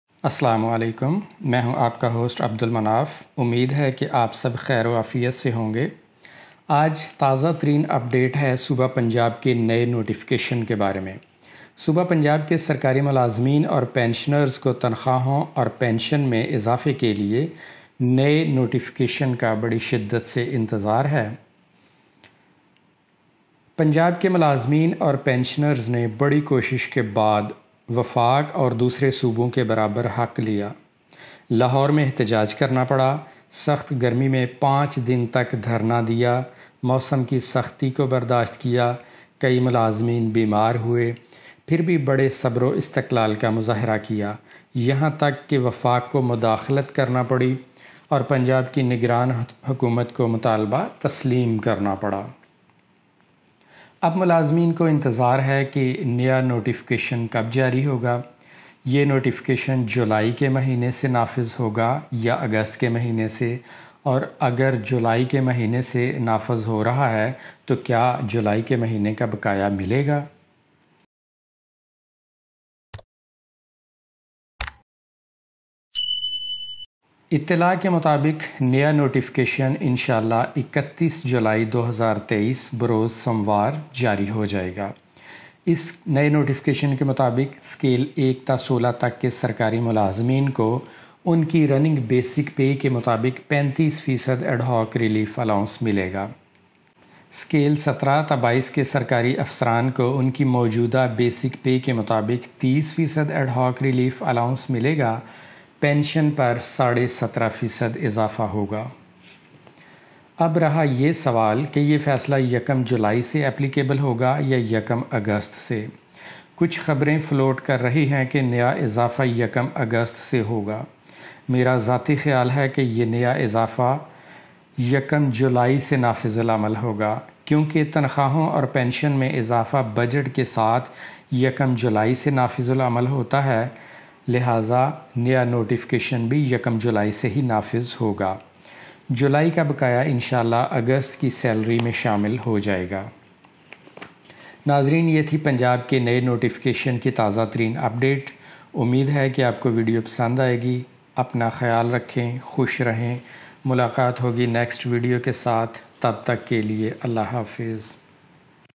Latest Update About Issuance of Revised Notification of Increase in Pay and Pension
Revised notification of an increase in pay and pension will be issued within a couple of days. Govt of Punjab is going to issue a new notification of increase in pay and pension of the employees. It great news for Govt servants that Govt of Punjab has increased the pay and pension equivalent to the Federal Govt and other employees of other provinces of Pakistan. It is a piece of good news that the revised notification of increased pay and pension is going to be issued.
Great news for govt employees and pensioners about their pay and pension notification